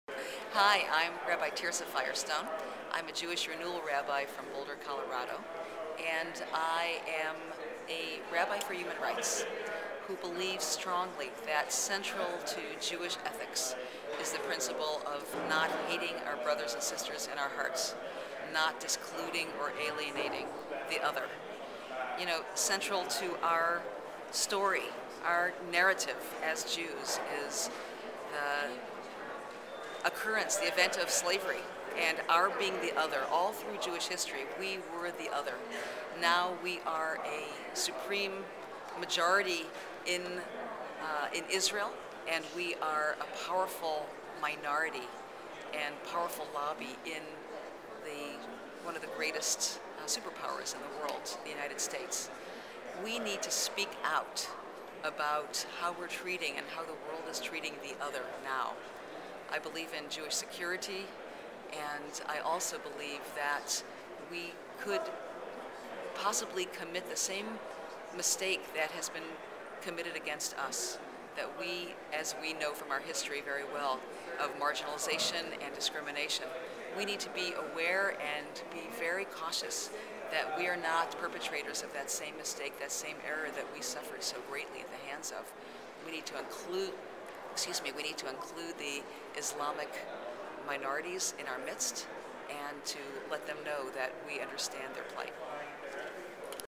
Stand Together: Rabbi Tirzah Firestone speaks out against Islamophobia
Rabbis for Human Rights-North America encourages rabbis and lay Jewish activists to STAND TOGETHER with their Muslim neighbors and friends, and speak out against anti-Muslim bigotry.  In this video, Rabbi Tirzah Firestone explains why Islamophobia is against Jewish values. You can find more videos from our campaign Join us by uploading your own video and simply include "rabbisagainstislamophobia" as one of the tagged keywords.